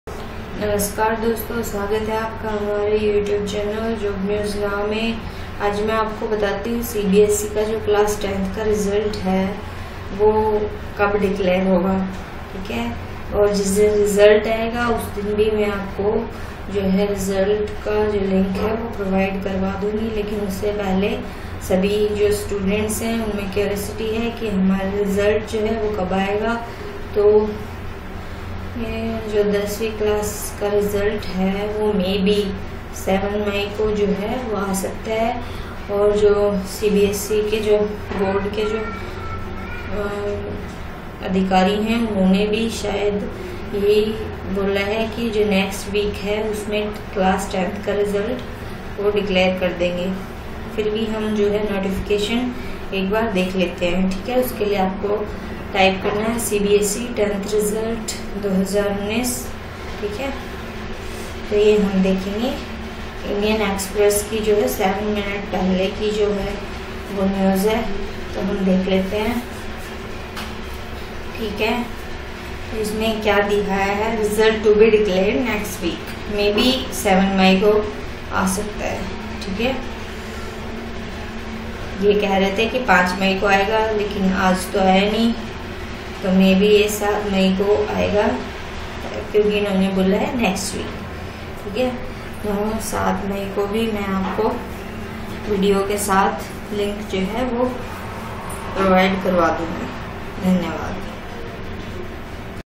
CBSE CLASS 10TH RESULT DATE NOTIFICATION,CBSE CLASS 10TH RESULT DATE 2019
Hello friends

CBSE Class 10th Result 2019 LIVE Updates: Results to be declared next week, to be available at cbse.nic.in, cbseresults.nic.in

The Central Board of Secondary Education will announce the result of Class 10 examination in the second week of May. Though various media portals are claiming that the results will be declared on May 5, 2019, CBSE secretary Anurag Tripathi said, “The results of Class 10 examination will be declared next week.” He, however, did not mention date of the announcement of results.